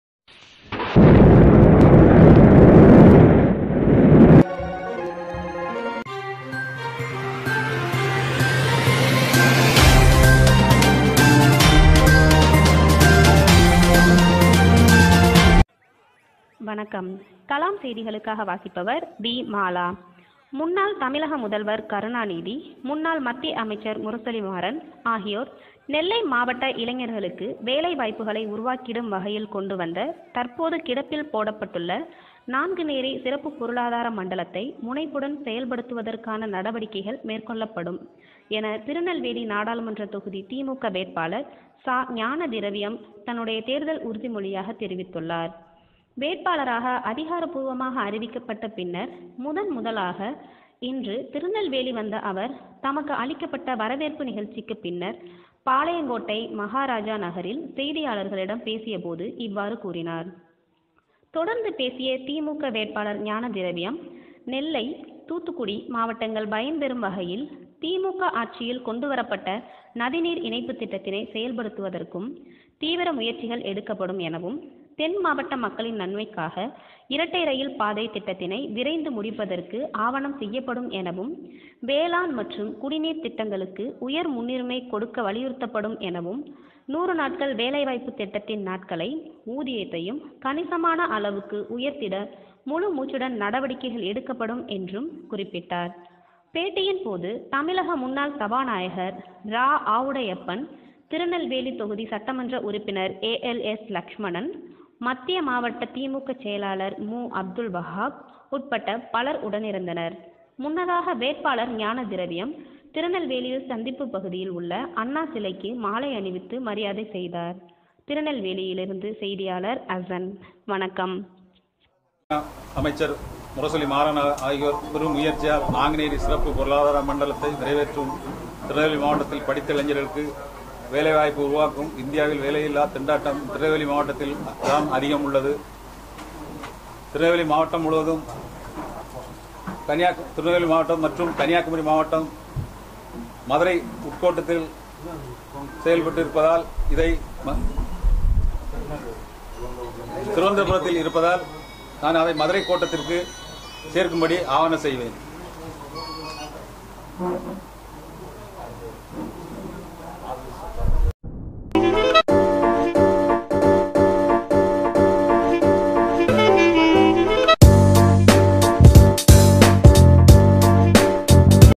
KALAM NEWS திருநெல்வேலி நாடாளுமன்ற தொகுதி திமுக வேட்பாளர், ஞான திரவியம், தன்னுடைய தேர்தல் உ
KALAM NEWS
திருநெல்வேலி நாடாளுமன்ற தொகுதி திமுக வேட்பாளர்,  ஞான திரவியம்,  தன்னுடைய தேர்தல் உறுதிமொழி